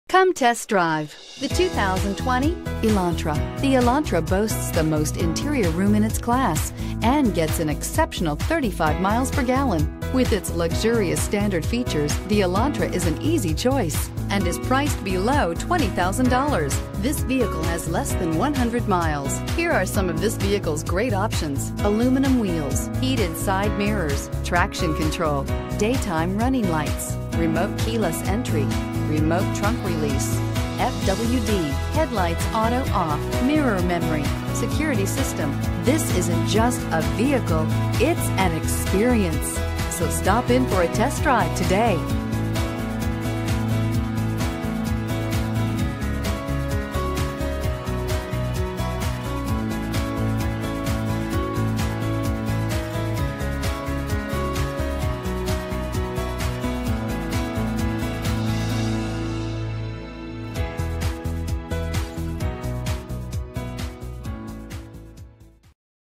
2020 Hyundai Elantra Spartanburg SC H200555
2020 Hyundai Elantra Value Edition

Benson Hyundai
2550 Reidville Road

$3,139 below Invoice! Priced below KBB Fair Purchase Price! Phantom Black 2020 Hyundai Elantra Value Edition FWD IVT 2.0L 4-Cylinder DOHC 16V 30/40 City/Highway MPGbrbr30/40 City/Highway MPG